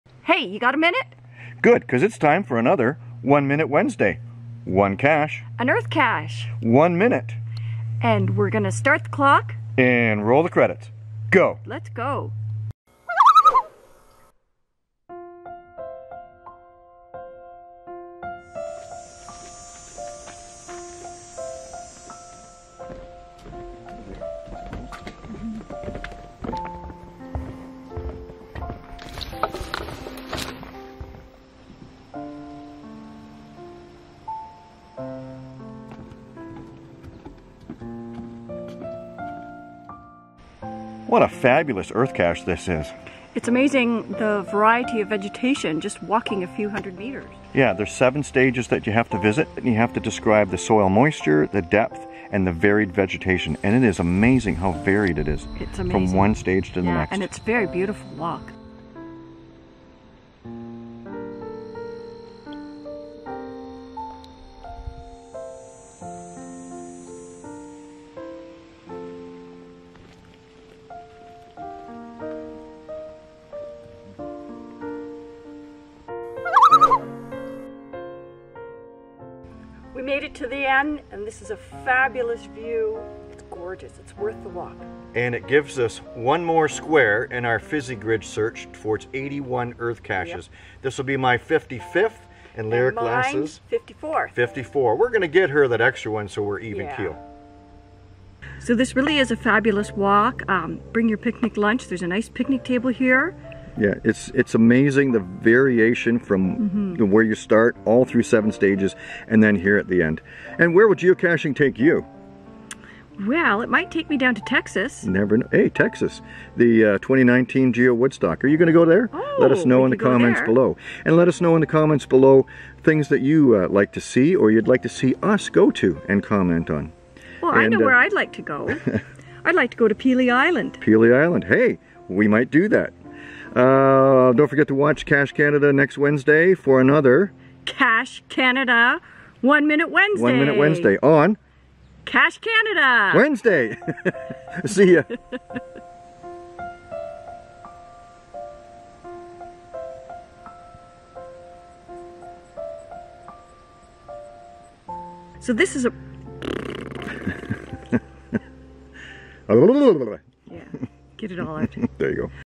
Geocaching - One Minute Wednesday - Amazing EC Walk
We targeted an Earthcache for its D/T rating but did not expect how amazing the walk through the different stages would be. Such a fabulous walk with different types of vegetation and terrain in such a short distance and so many insect and plant varieties. Come along for this episode of One Minute Wednesday.